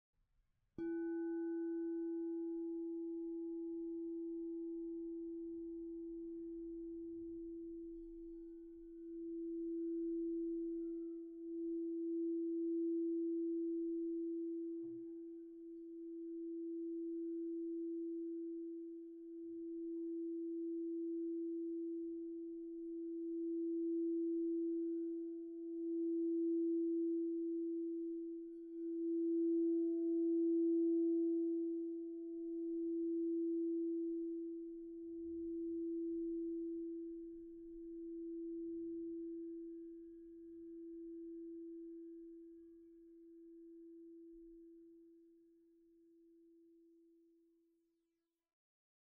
Sonido Cuenco de Cuarzo - Nota MI - Tamaño 8" de diámetro
Cuenco de cuarzo nota Mi en 8 pulgadas de diámetro.

Escucha nuestros cuencos de cuarzo en todas las notas.

UniSono